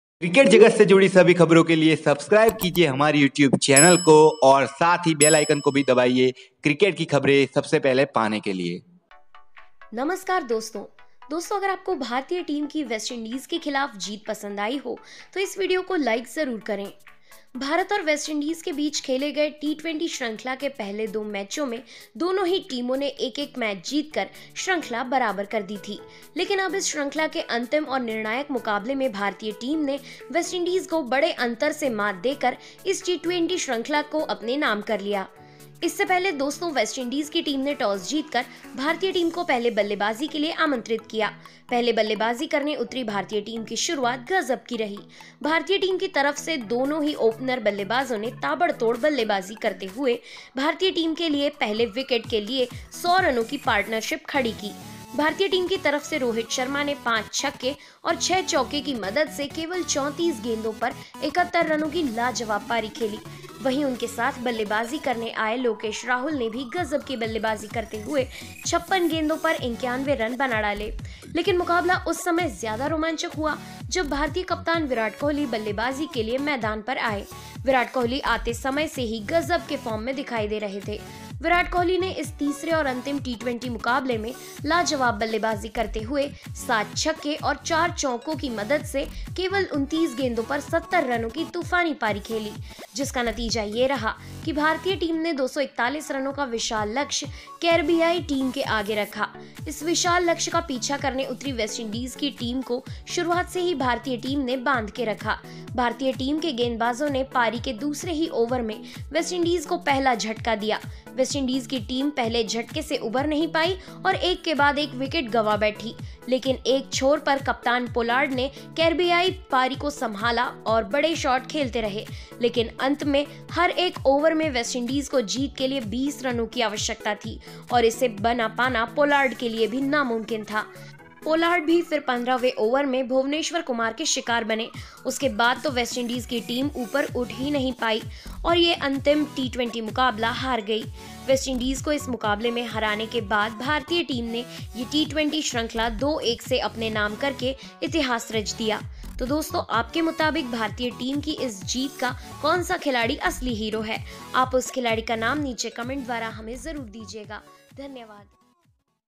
𝗛𝗜𝗚𝗛𝗟𝗜𝗚𝗛𝗧𝗦 : तीसरे टी20 मे भारत ने रौंदा वेस्टइंडीज को 2-1 से किया सीरीज पर कब्जा
भारत और वेस्टइंडीज के बीच खेले गए टी-20 श्रृंखला के पहले दो मैचों में दोनों ही टीमों ने एक-एक मैच जीतकर श्रृंखला बराबर कर दी थी लेकिन
अब इस श्रृंखला के अंतिम और निर्णायक मुकाबले में भारतीय टीम ने वेस्टइंडीज को बड़े अंतर से मात देकर इस टी-20 श्रृंखला को अपने नाम कर लिया । इससे पहले दोस्तों वेस्टइंडीज की टीम ने टॉस जीतकर भारतीय टीम को पहले बल्लेबाजी के लिए आमंत्रित किया । पहले बल्लेबाजी करने उतरी भारतीय टीम की शुरुआत गजब की रही । भारतीय टीम की तरफ से दोनों ही ओपनर बल्लेबाजों ने ताबड़तोड़ बल्लेबाजी करते हुए भारतीय टीम के लिए पहले विकेट के लिए 100 रनों की पार्टनरशिप खड़ी करी